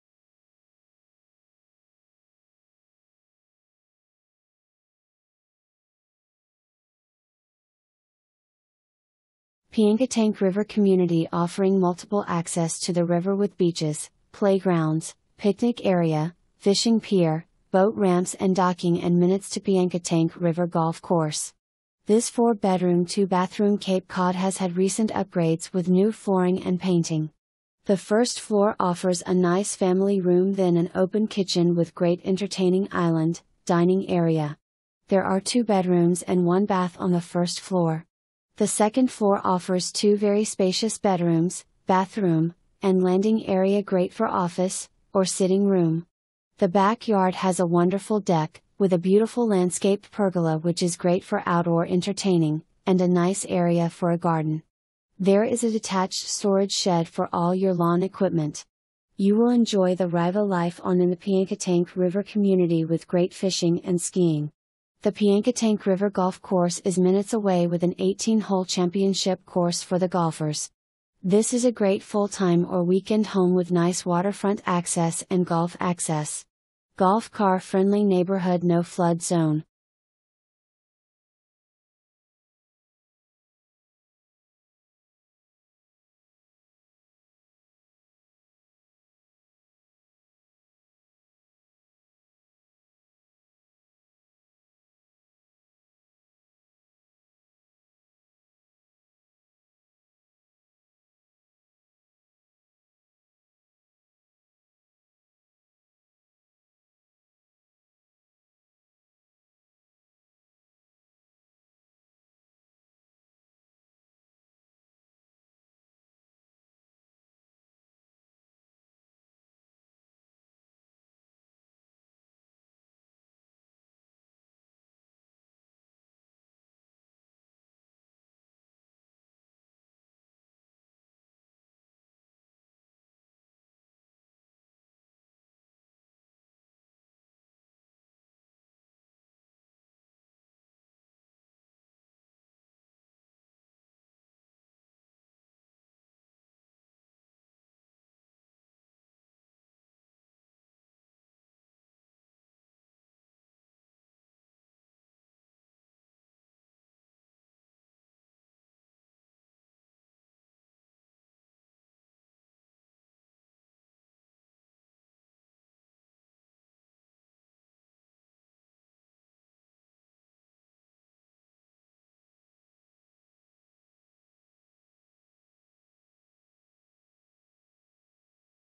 738 Coachpoint Road
738 Coachpoint Road
Hartfield, VA 23071

Piankatank River Community offering multiple access to the River with beaches, playgrounds, picnic area, fishing pier, boat ramps and docking and minutes to Piankatank River Golf Course. This 4 bedroom 2 bathroom Cape Cod has had recent upgrades with new flooring & painting. The first floor offers a nice family room then an open kitchen with great entertaining island, dining area. There are 2 bedrooms and 1 bath on the first floor. The second floor offers 2 very spacious bedrooms, bathroom, and landing area great for office, or sitting room. The back yard has a wonderful deck, with a beautiful landscaped pergola which is great for oudoor entertaining, and a nice area for a garden. There is a detached storage shed for all your lawn equipment. You will enjoy the Rivah life on in the Piankatank River community with great fishing and skiing. The Piankatank River Golf course is minutes away with an 18 hole championship course for the golfers!  This is a great full time or weekend home with nice waterfront access and golf access.  Golf car friendly neighborhood No Flood Zone!

2 full bath

Renee Edwards
Morgan & Edwards Real Estate, LLC